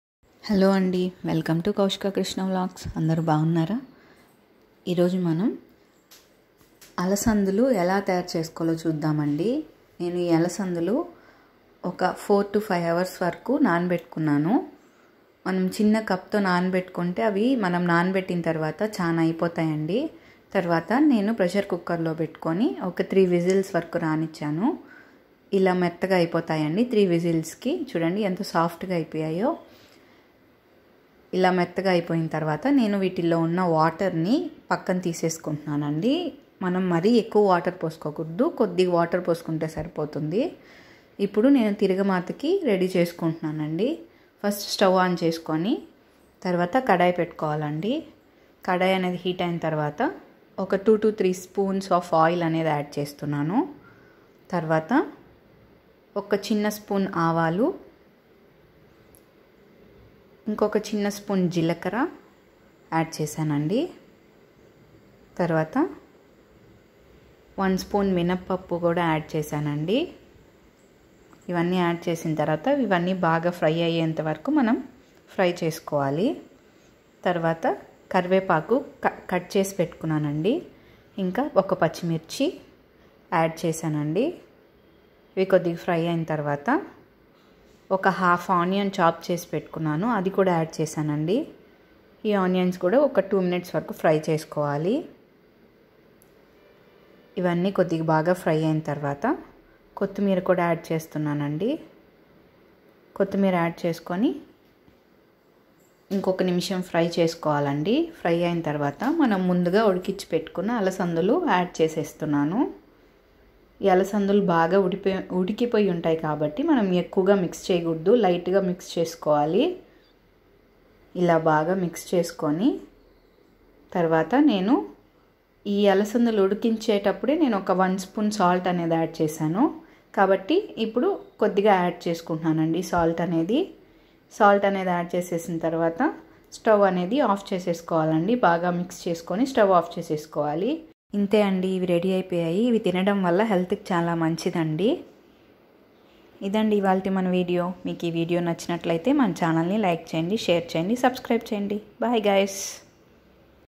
Alasandhulu🥣 || evvi thinta chaala strength pillalaku vetilne snacks laaga evng time lo petandi
very strengthy and tasty snack